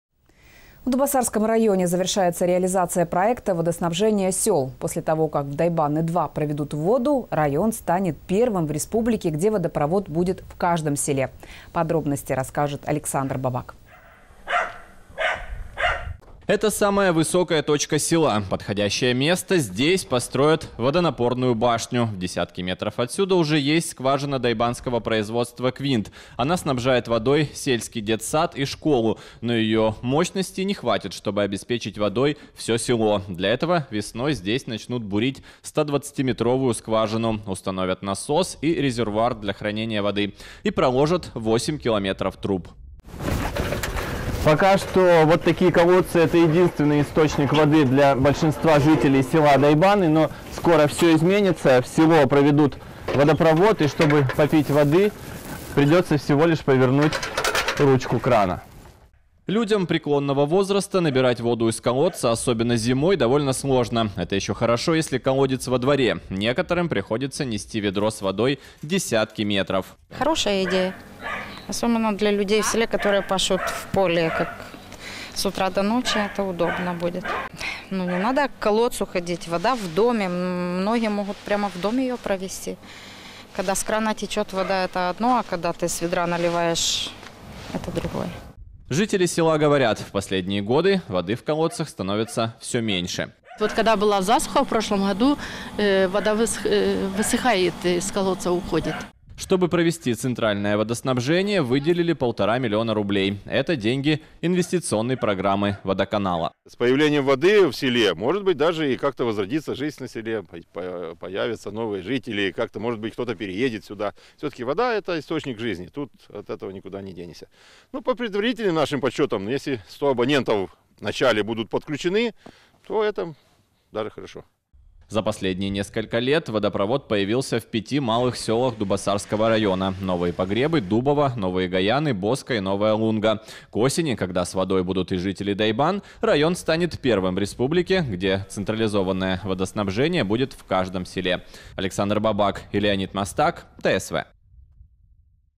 Вода в Дойбаны-2
В Дубоссарском районе завершается реализация проекта водоснабжения сёл. После того как в Дойбаны-2 проведут воду район станет первым в республике, где водопровод будет в каждом селе. Подробности расскажет Александр Бабак.